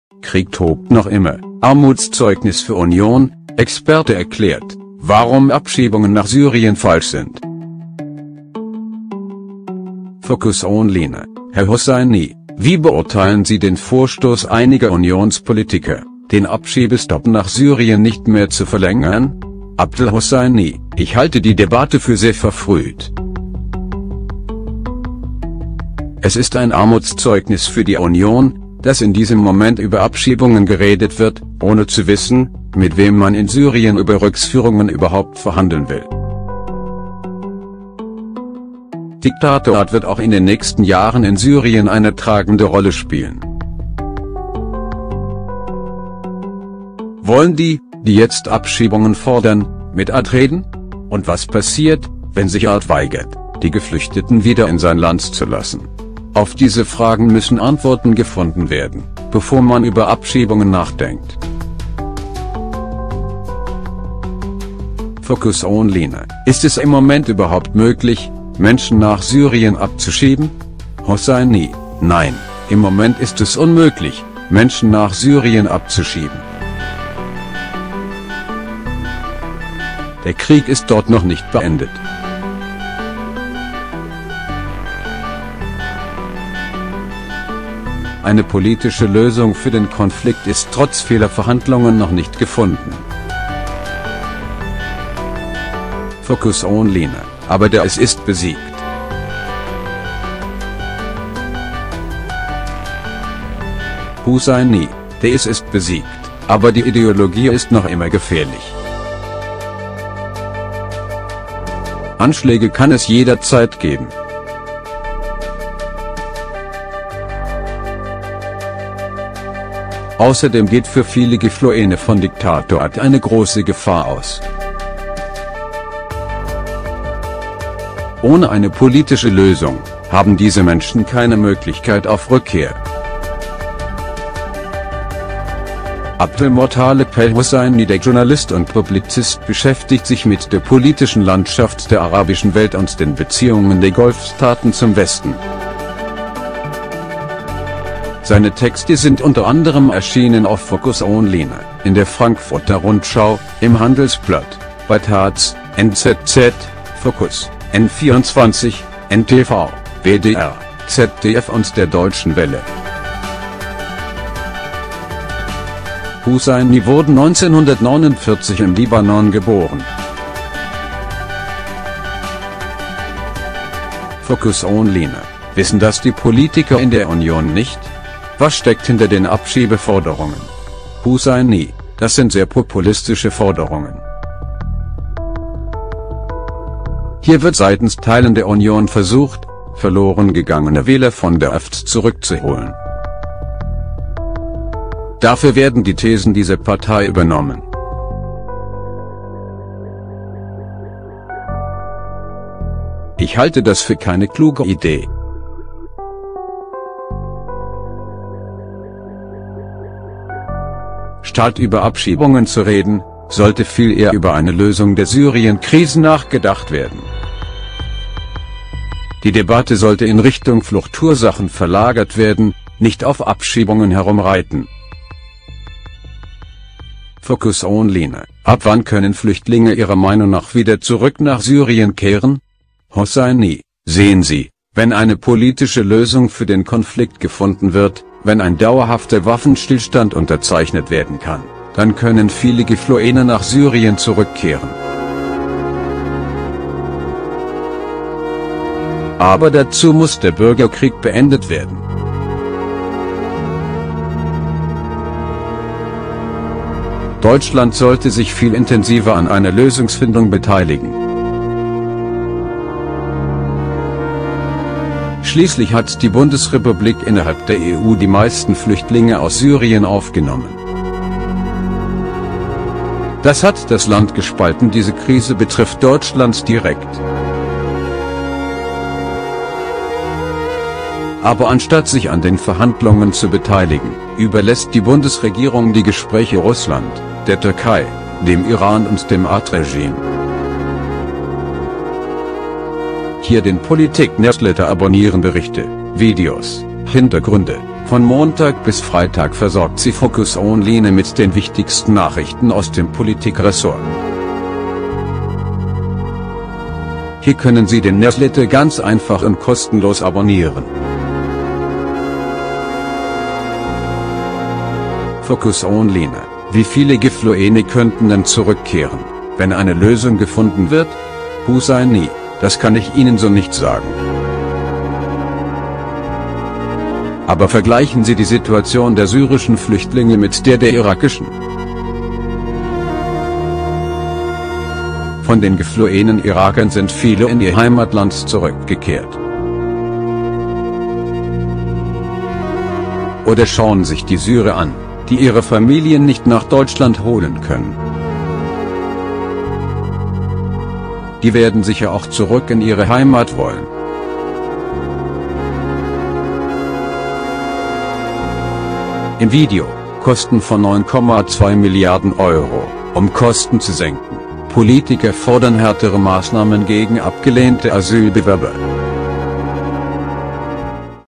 Krieg tobt noch immer: "Armutszeugnis für Union": Experte erklärt, warum Abschiebungen nach Syrien
Die Innenminister der unionsgeführten Bundesländer Sachsen und Bayern wollen den Abschiebestopp für Syrer nur noch bis Mitte 2018 verlängern. Danach sollen syrische Flüchtlinge wieder in das vom Bürgerkrieg gebeutelte Land abgeschoben werden können. Nahost-Experte Abdel Husseini erklärt, warum das Vorhaben derzeit unmöglich ist.